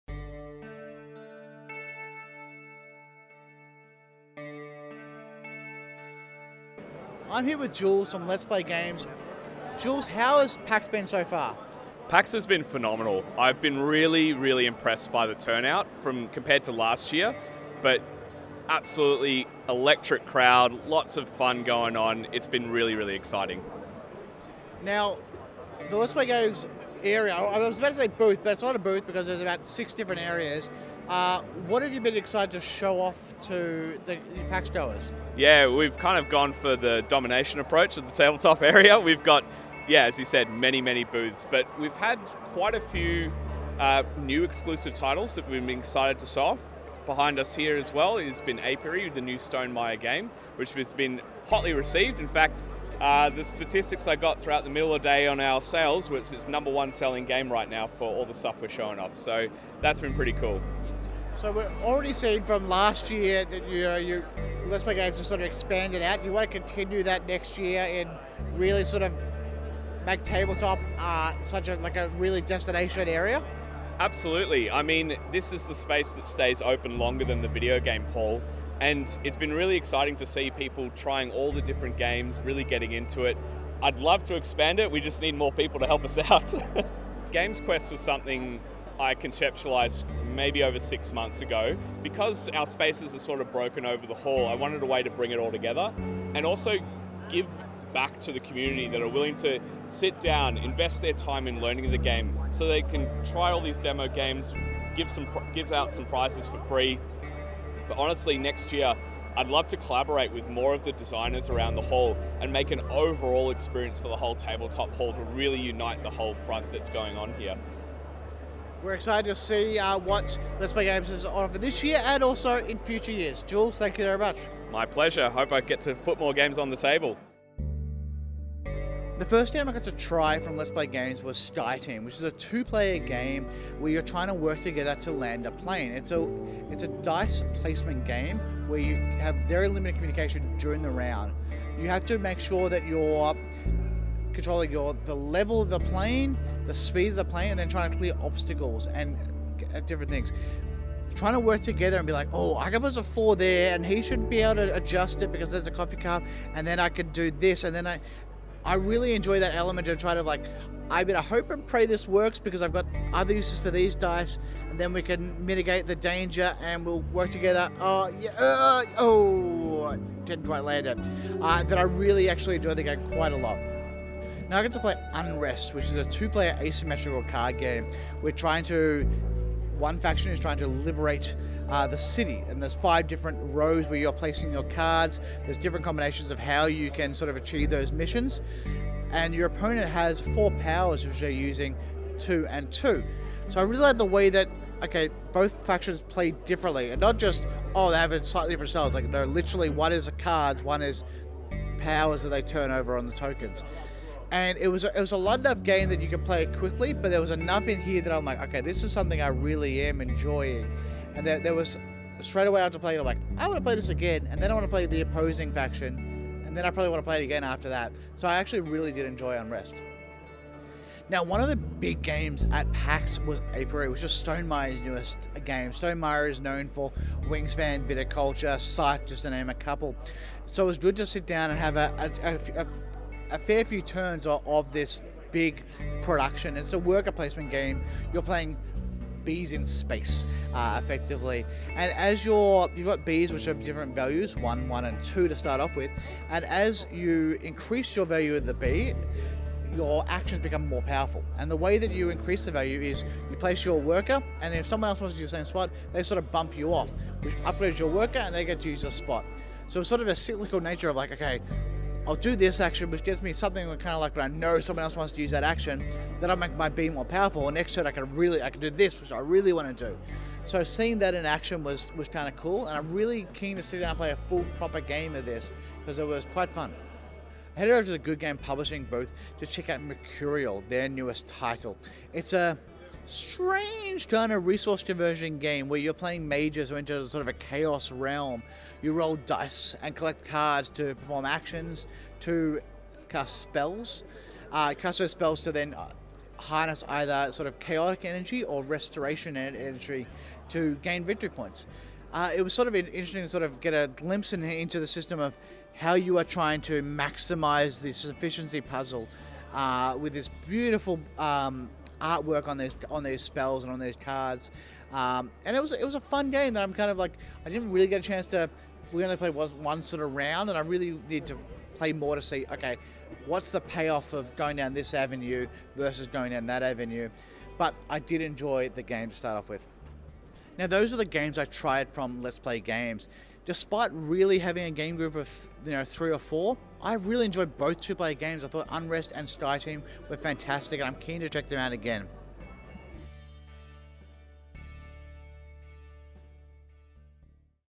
PAX AUS 23: Let's Play Games Demos
Liam chats to Jules from Let's Play Games about their board game offerings and Game Quest at PAX. Plus, he tries four demos and gives his first impressions of Sky Team, Unrest, Apiary and Mercurial.